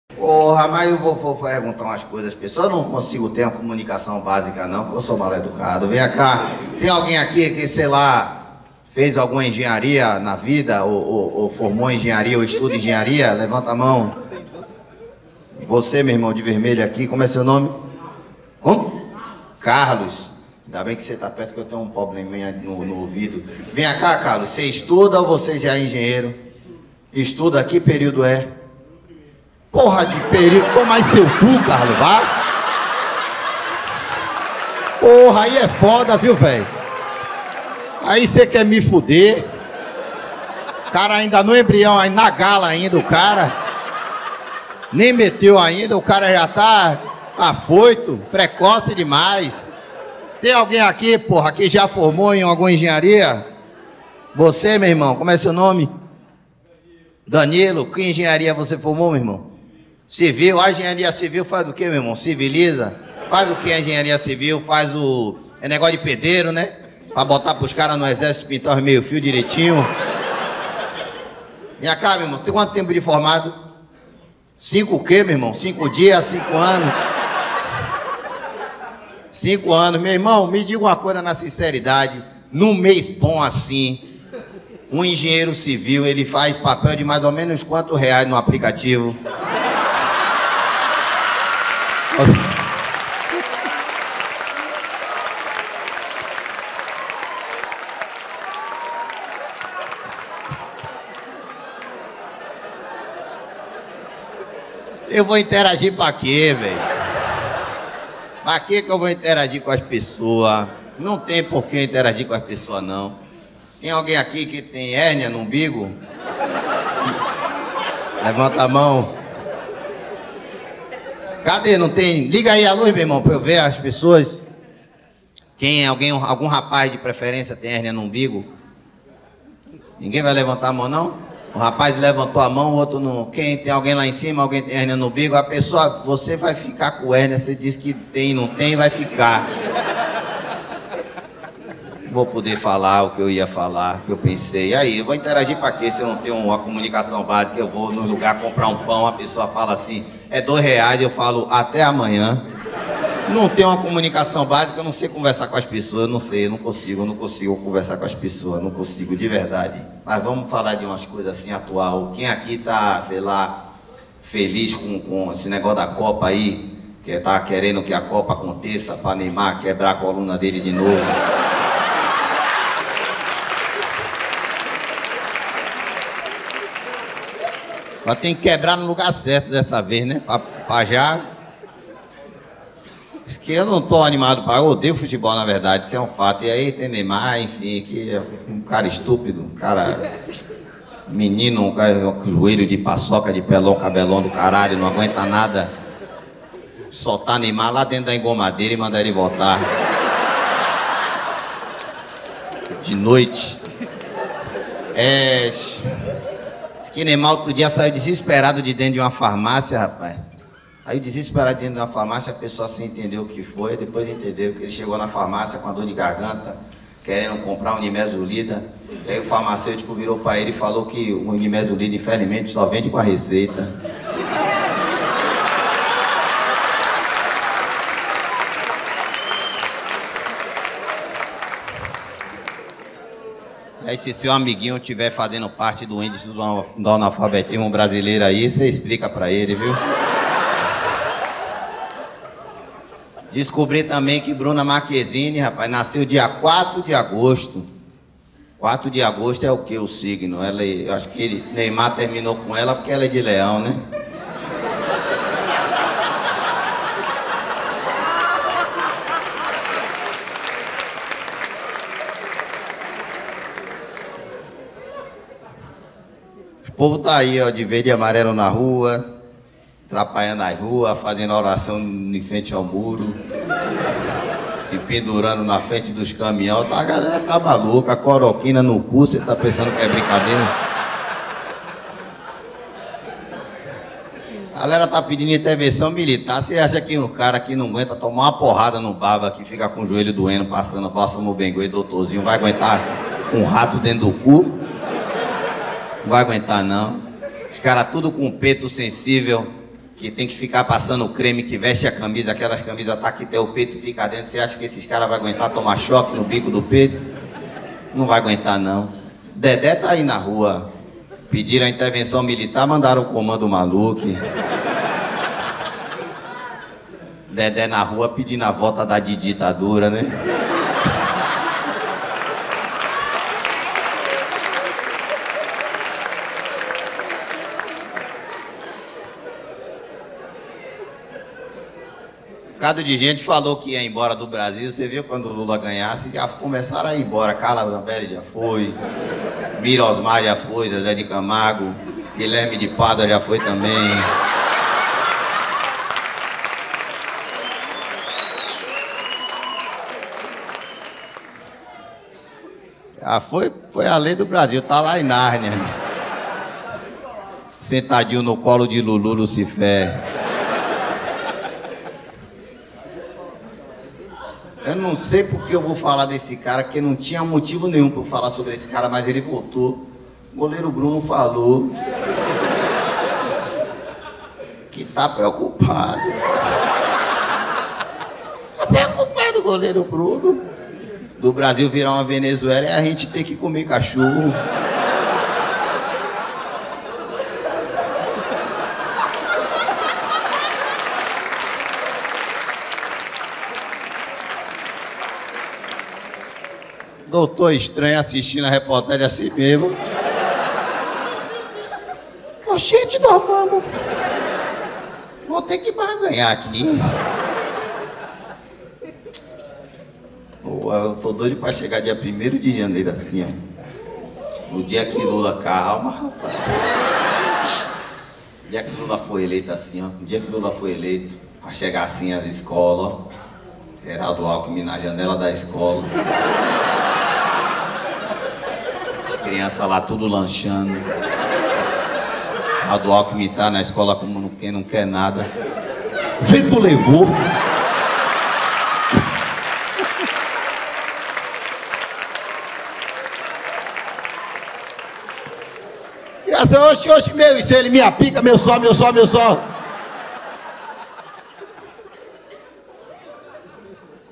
OS PATRIOTAS ESTÃO CHEGANDO - STAND UP COMEDY - JOÃO PIMENTA
Parafraseando inversamente proporcional Jorge Ben: Eles estão chegando e vão dominar o mundo pela quantidade de idiotas.